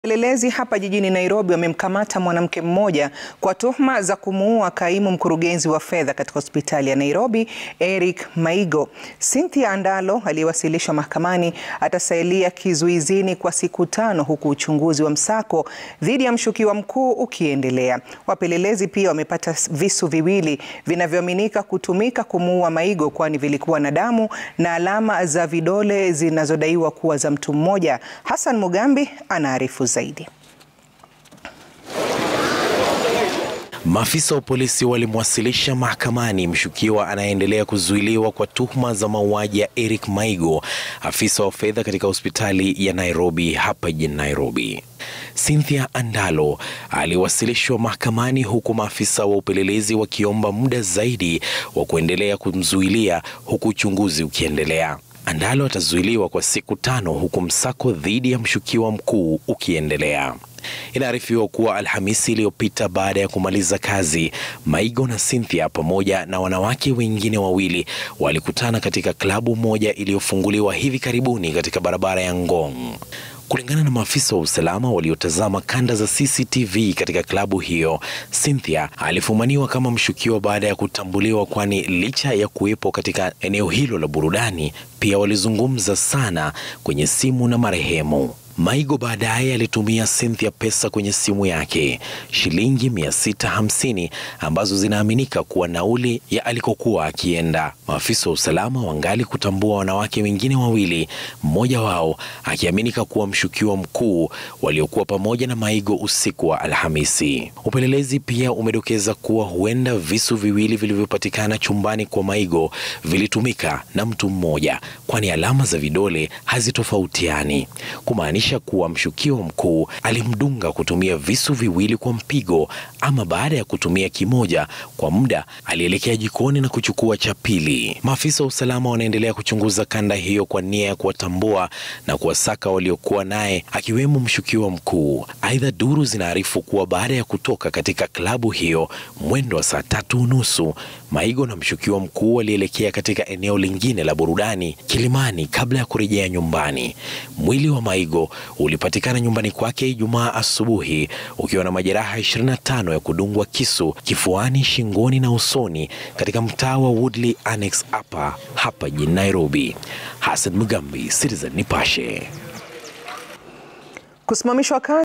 Mwanamke mmoja anazuiliwa kwa tuhuma za mauaji
Wapelelezi jijini Nairobi wamemkamata mwanamke mmoja kwa tuhuma za kumuua kaimu mkurugenzi wa fedha katika hospitali ya Nairobi Eric Maigo. Cynthia Andalo aliyewasilishwa mahakamani atasalia kizuizini kwa siku tano huku uchunguzi na msako dhiidi ya mshukiwa mkuu ukiendelea.wapelelezi pia wamepata visu viwili vinavyoaminika kutumika kumuua maigo kwani vilikuwa na damu na alama za vidole zinazodaiwa kuwa za mtu mmoja